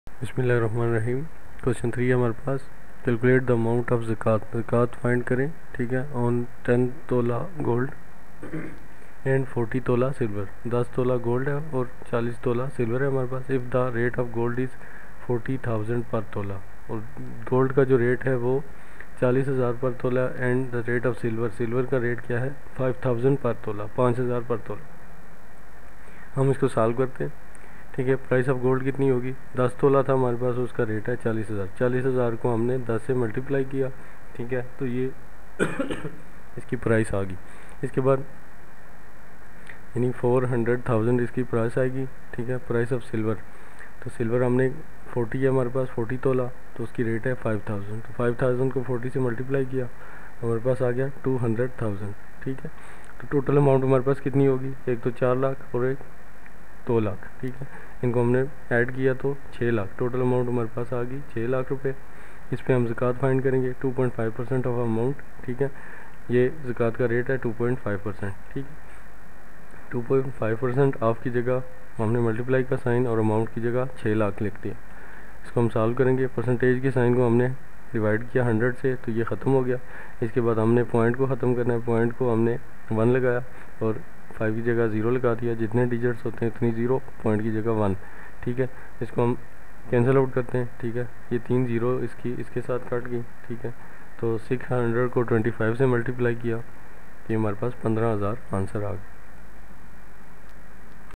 9th General Math Exercise 2.1 Question 3 || class 9
Q3: Calculate the amount of Zakat on 10 tola gold and 40 tola silver, if the rate of gold is Rs 40,000 per tola and the rate of silver is Rs 5000 per tola.

Student9001,9th,class,general group,mathematics,exercise 2.1,question 3, 9th general math exercise 2.1 question 3 , smart thinker club